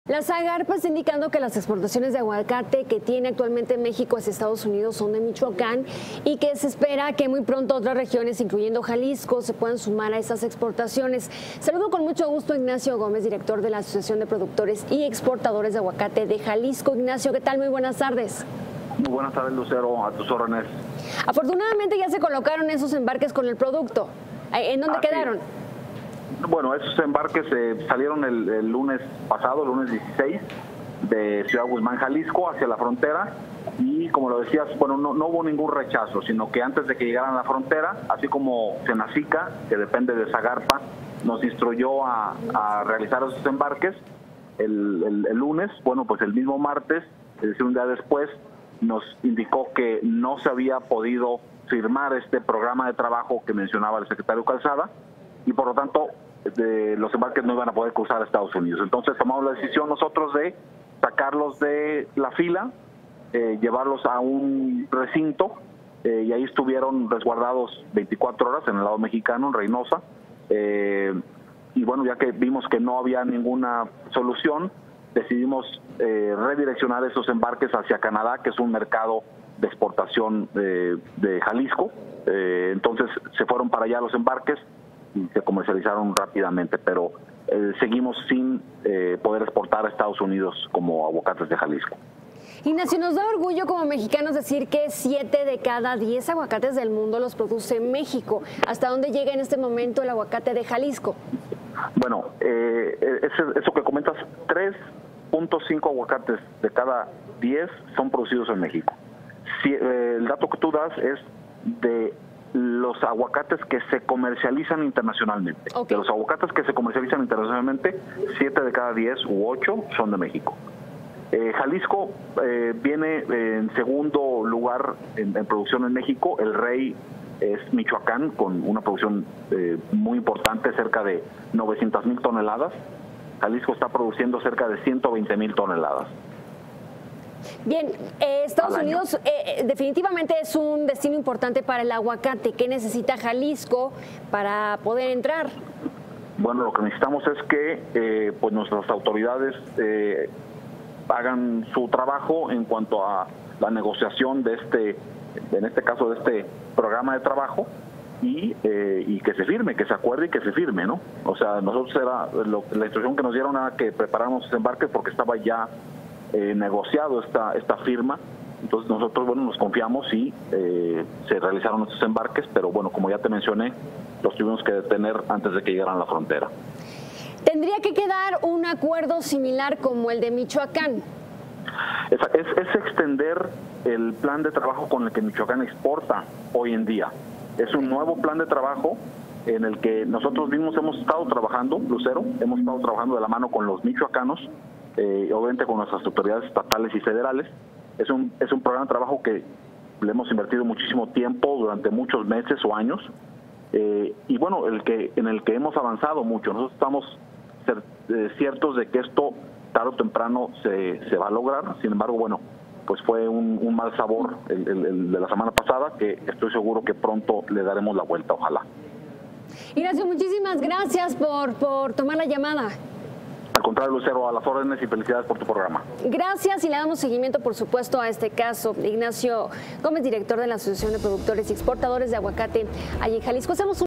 Aguacate retenido en la frontera se redireccionó a Canadá: Asociación de productores de Aguacate
Ignacio Gómez, director de la Asociación de Productores y Exportadores de Aguacate de Jalisco. Tema: frenan cargamento de aguante a Estados Unidos